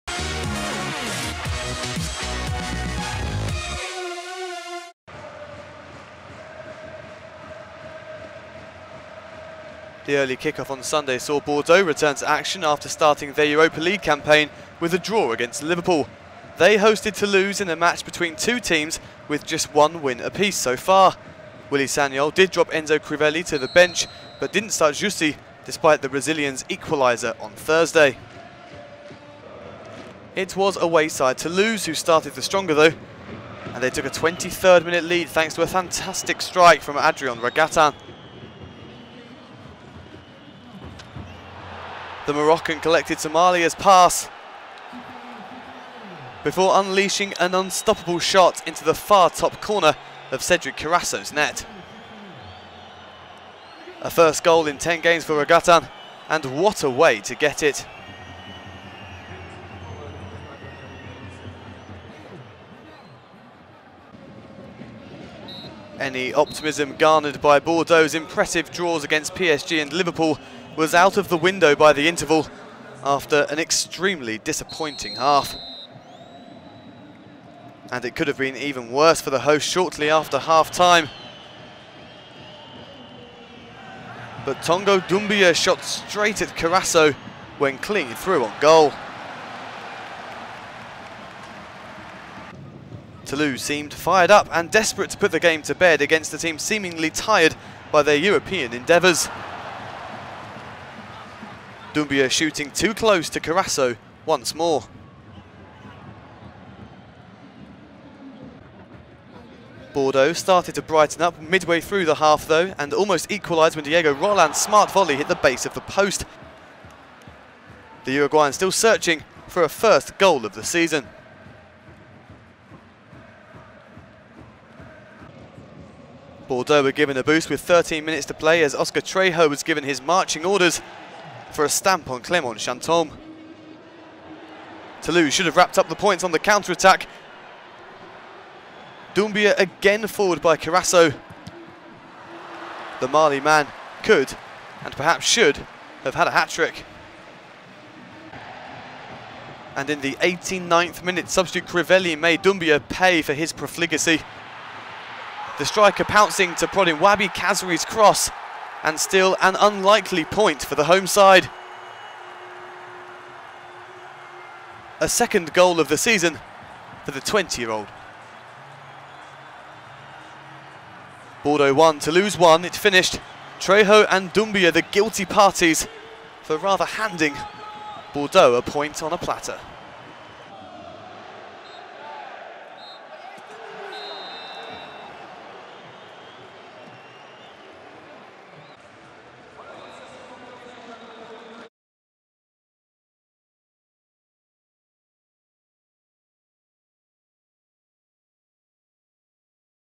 Girondins de Bordeaux - Toulouse FC (1-1) - Highlights - (GdB - TFC) / 2015-16
Girondins de Bordeaux vs Toulouse FC (1 - 1) highlights. The best actions and goals of Girondins de Bordeaux vs Toulouse FC in video.

Matmut Atlantique - Sun, 20 Sep 2015

Goals : Adrien REGATTIN (23' - Toulouse FC) / Enzo CRIVELLI (89' - Girondins de Bordeaux)

Red cards : Oscar TREJO (77' )

Toulouse FC Team line-up : 1 - Mauro GOICOECHEA / 3 - Jean-Armel KANA-BIYIK / 4 - Tongo Hamed DOUMBIA / 7 - Jean-Daniel AKPA-AKPRO (c) / 9 - Martin BRAITHWAITE / 17 - Adrien REGATTIN / 18 - Oscar TREJO / 19 - SOMALIA / 20 - Steeve YAGO / 23 - Yann BODIGER / 26 - Marcel TISSERAND